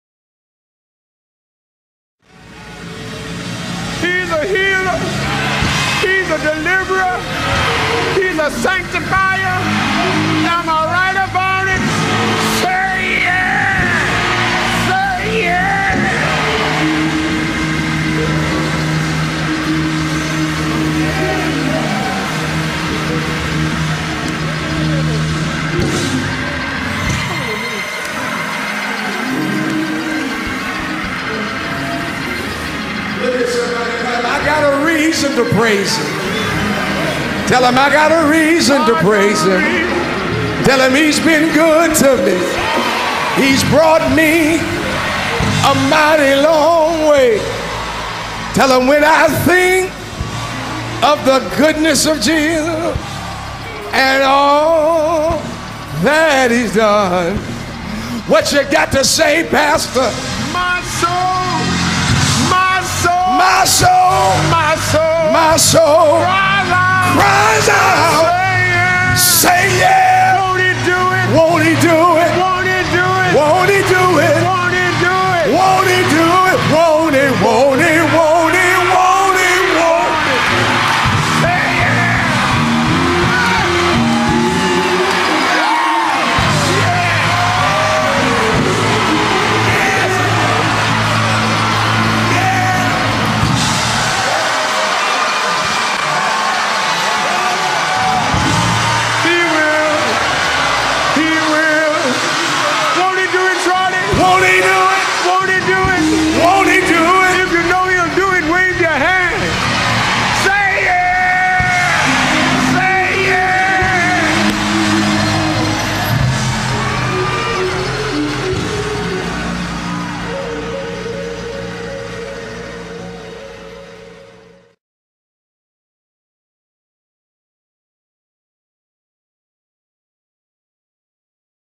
Bishop McKissick Sr. & Bishop McKissick, Jr. Preaching
Bishop Rudolph McKissick, Sr. preached his final pastoral sermon on Sunday, January 5, 2014. He enters retirement after serving as pastor for 47 years. This powerful moment happened during that final sermon.